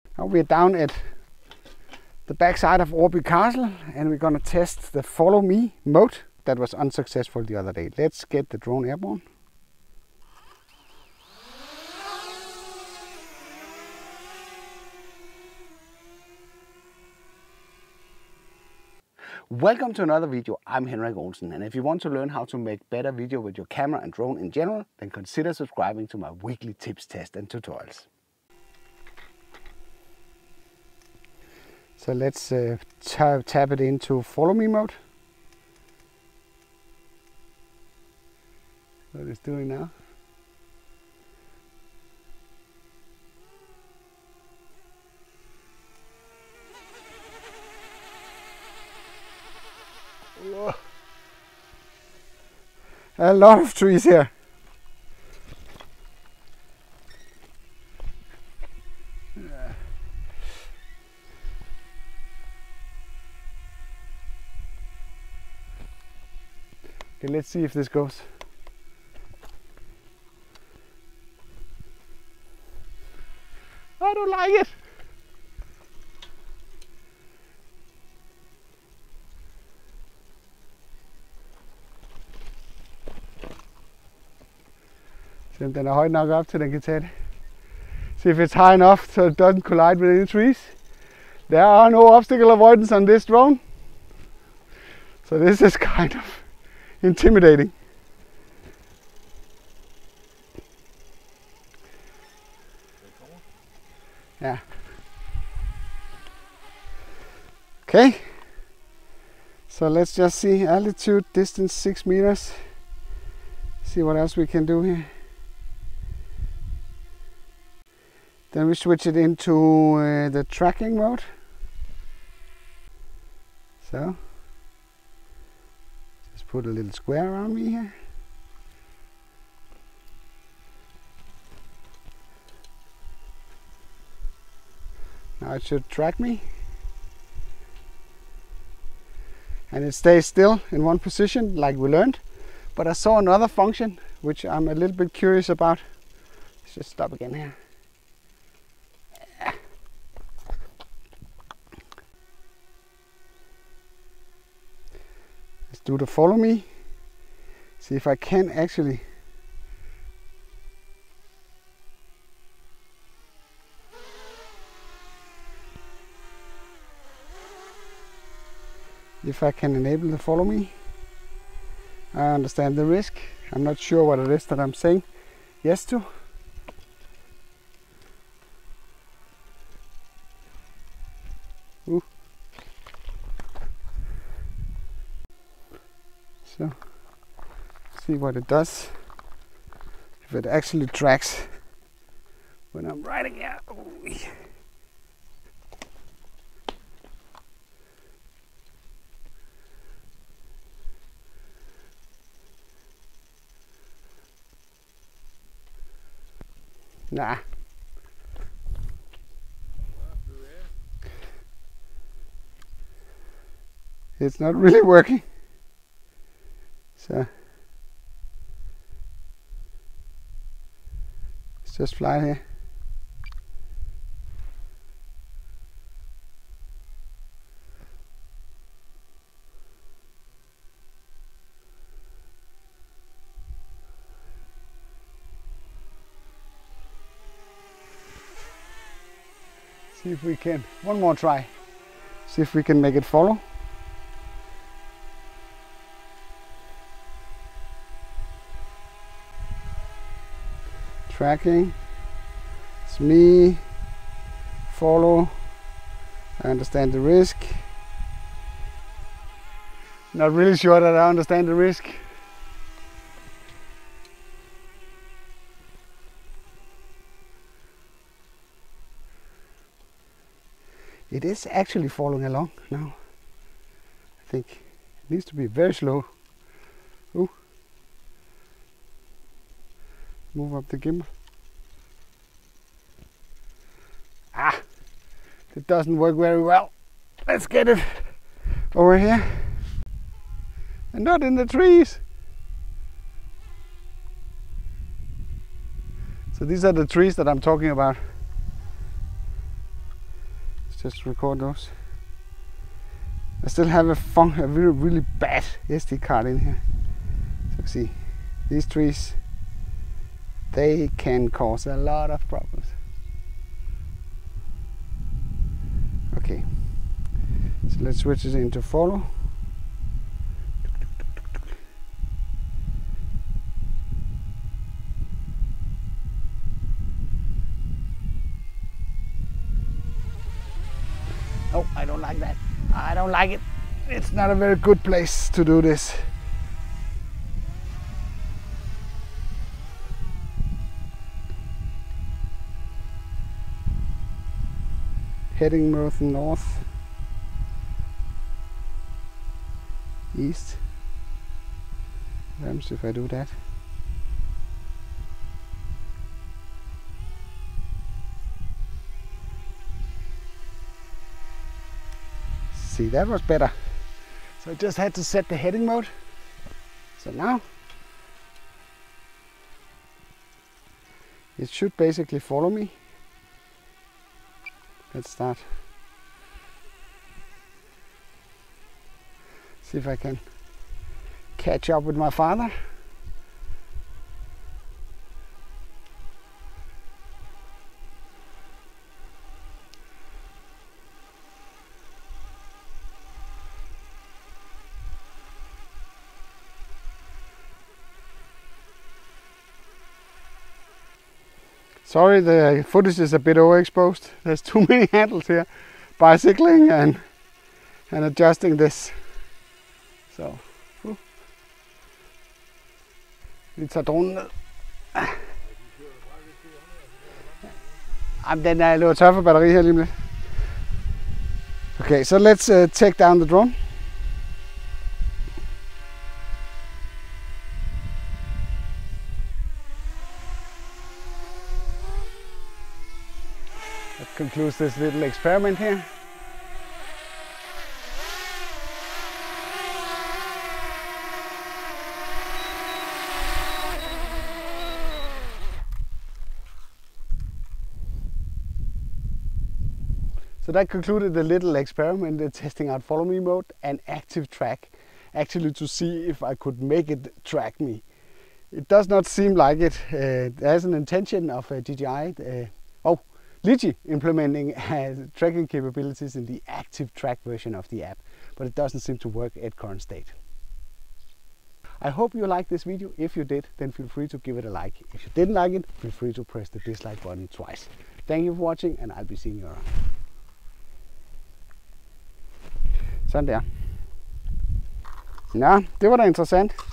DJI Mavic Mini ACTIVE TRACK with FOLLOW ME Mode Testing
Mavic Mini active track with follow me mode testing trying to track me while bicycling with my father. This is not a complete tutorial but just me messing around.

SanDisk High Endurance 256GB V30 microSDXC

SanDisk High Endurance 64GB V30 microSDXC

-- IMPORTANT --